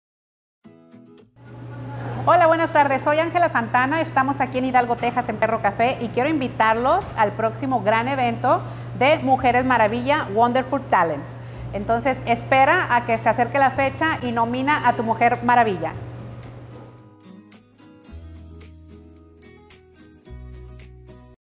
Nominación de la Mujer Maravilla 2022
Puedes mandar el testimonio de tu mujer maravilla . Evento en el Valle de Texas